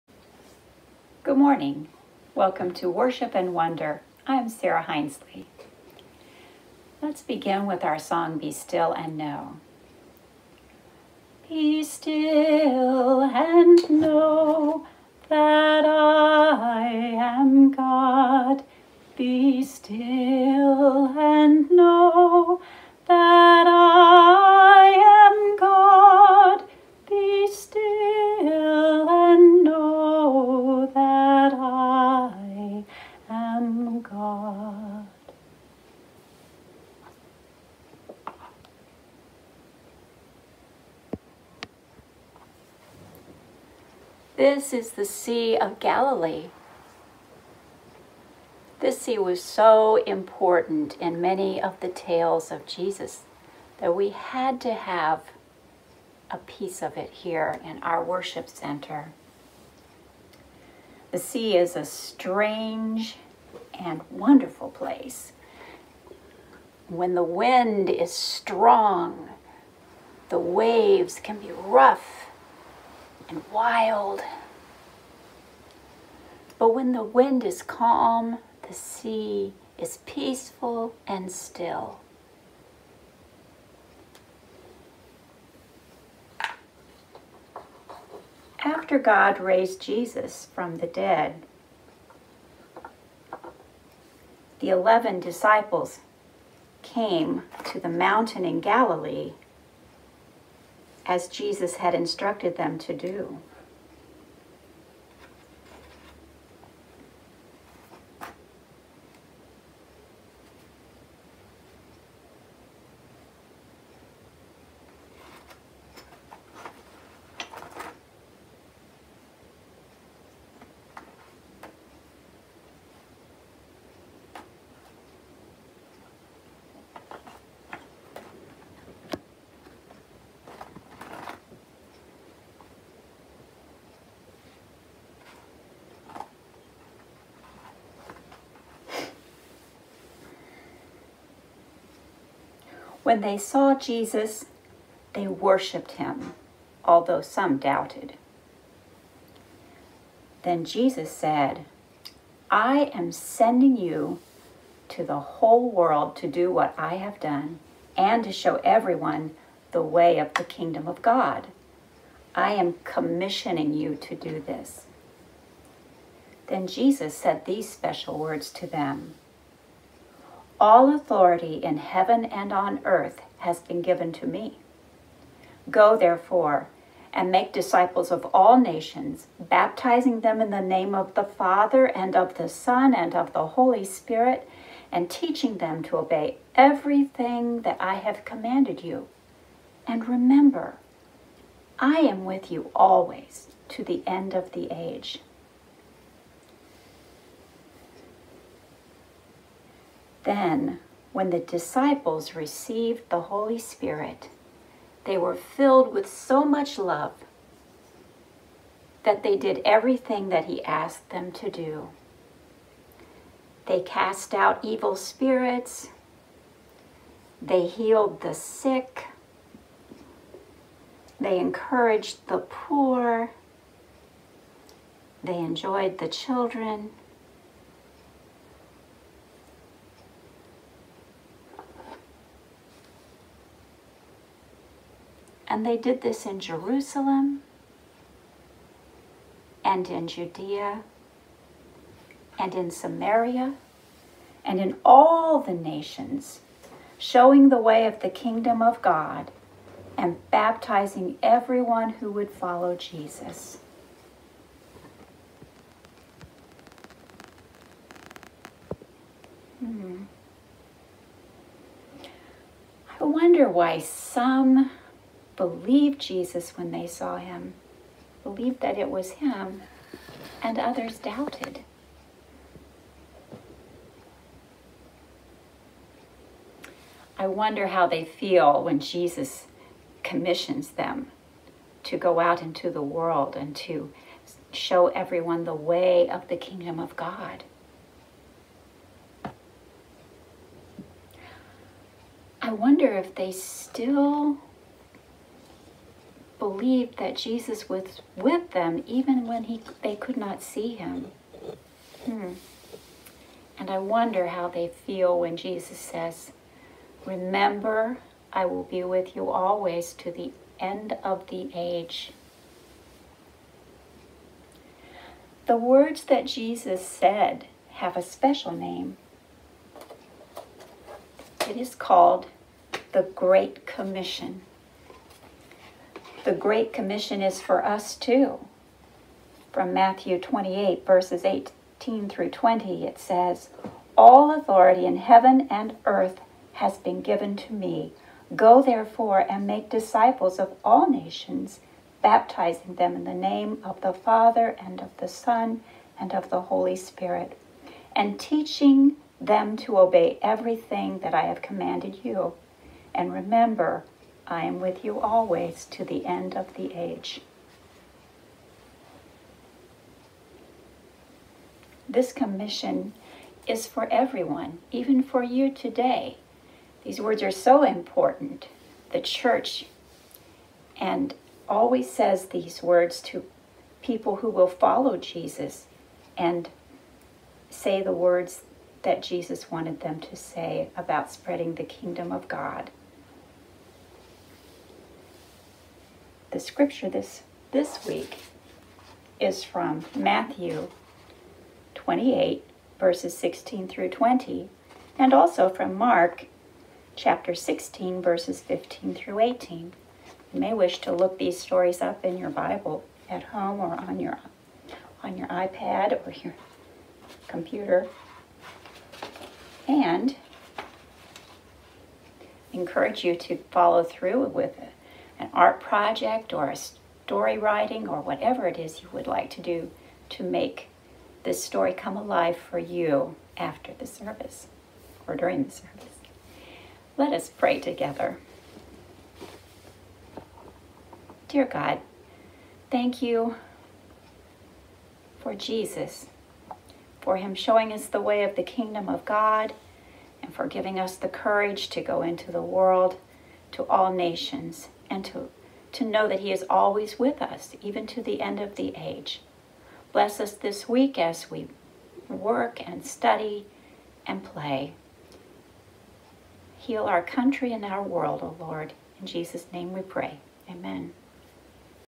Worship & Wonder: The Great Commission
Invite your children to gather close for this special story about the Great Commission, Jesus' command to his disciples to make disciples. What do you hear? I wonder how you would react to Jesus' invitation.